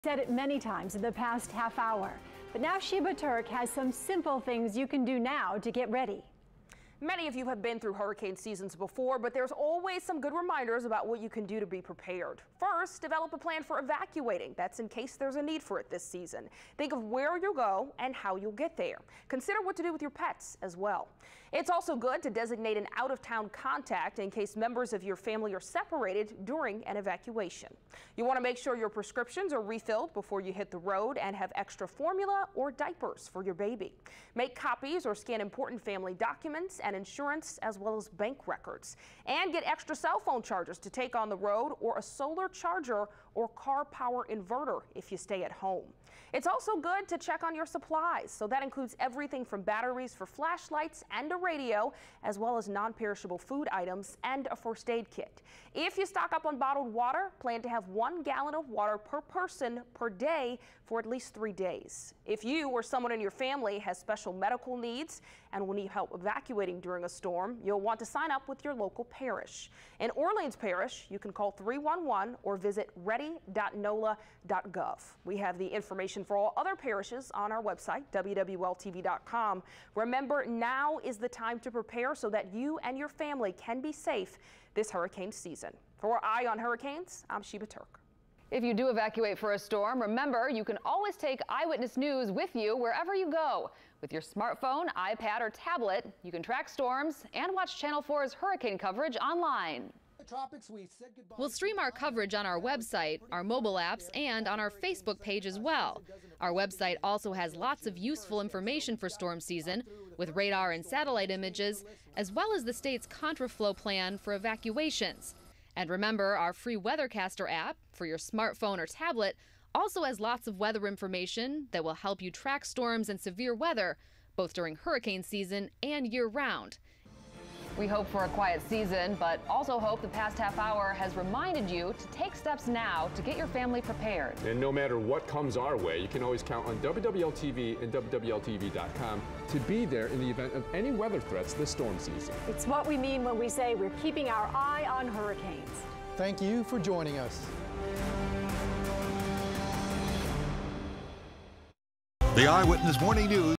Hurricane Special Part 4: Get prepared
We have some tips on what you need to have to be prepared in the event a storm comes our way.